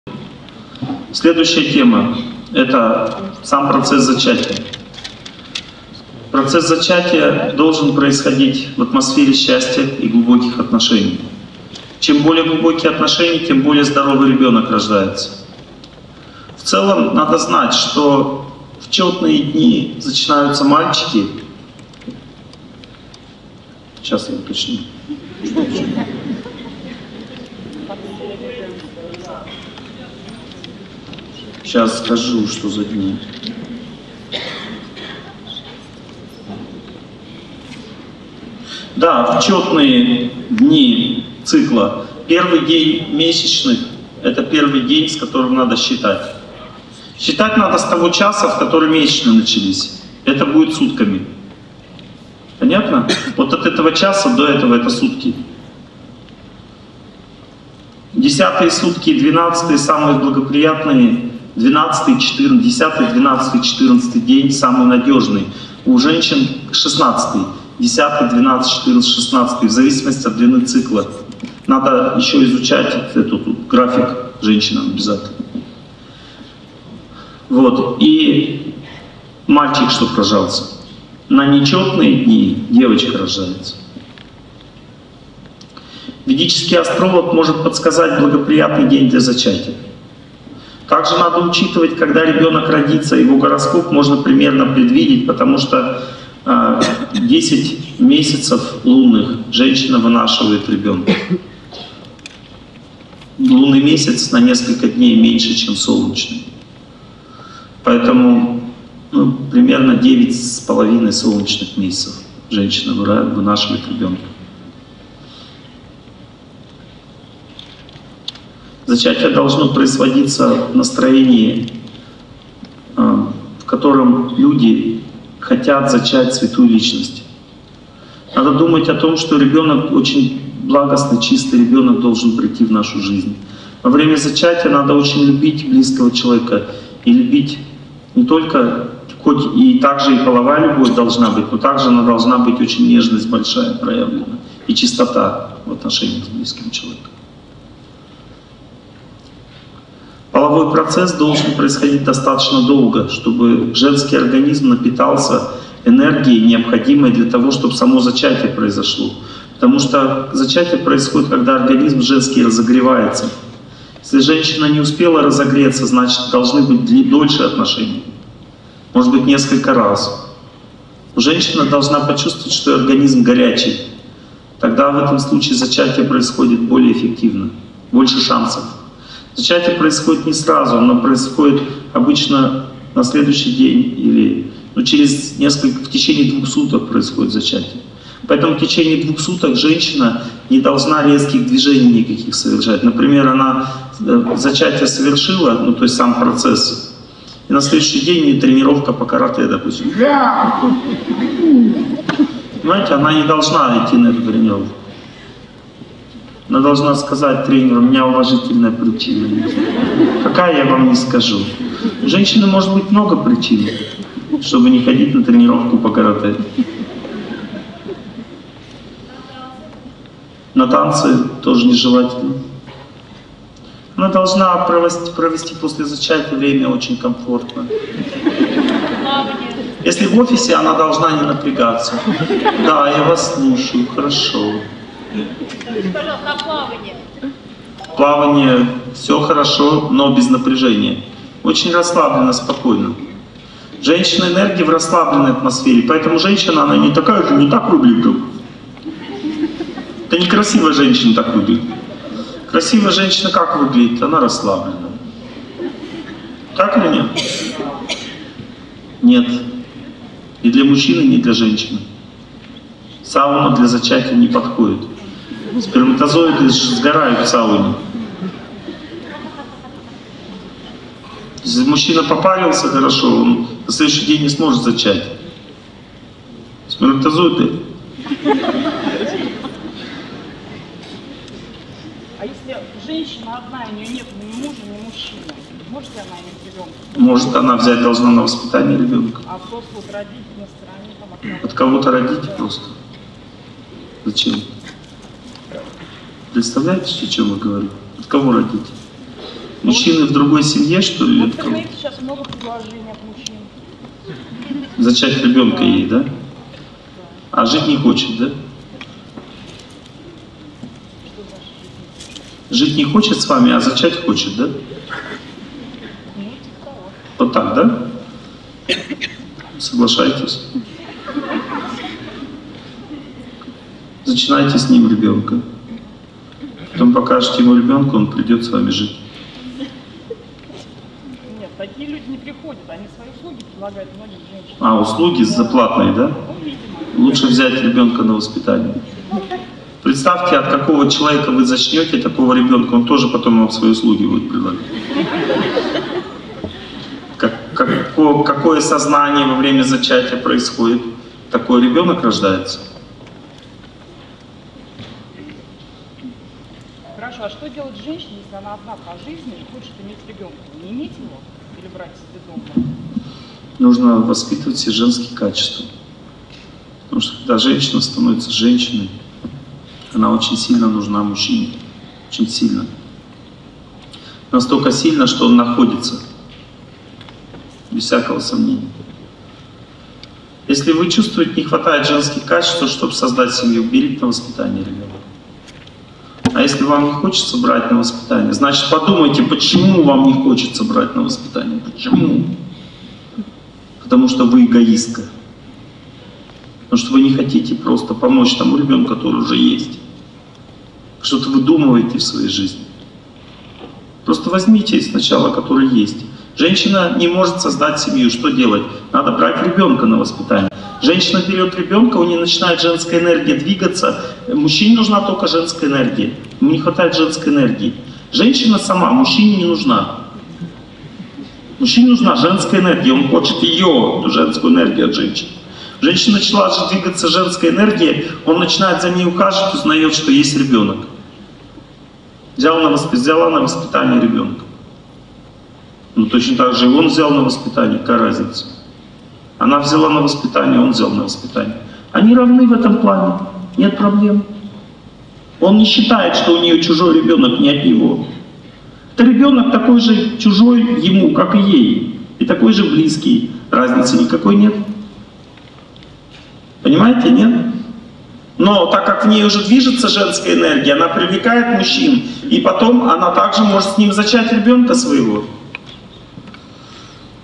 Торсунов О.Г. О процессе зачатия
В этом видео говорится о процессе зачатия
Из лекции "Таинство зачатия и рождения" Москва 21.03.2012